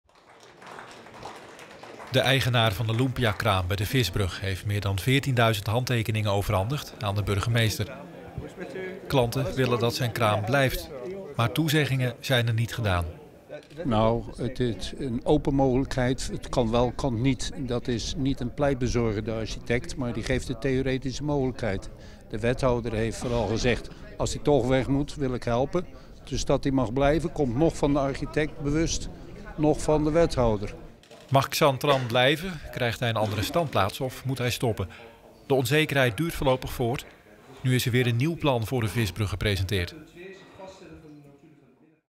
Duizenden handtekeningen tegen verhuizing loempiakraam in Dordrecht
NI20160413 loempiakraam